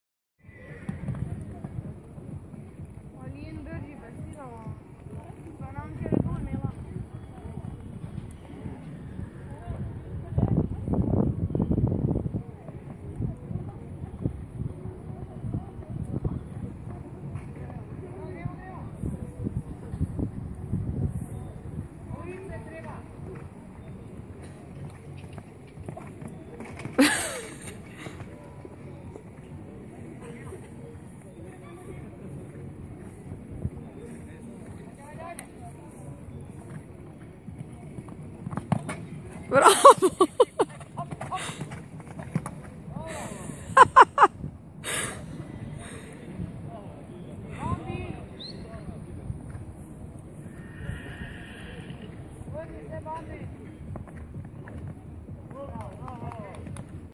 Parkur 80 cm Meljski hrib 23.11.2016
Kobila Tara je letnik 1998 in je odličen konj za svoja leta. Drugo leto jo bomo uporabljali za trening mladih jahačev na višini 50 cm.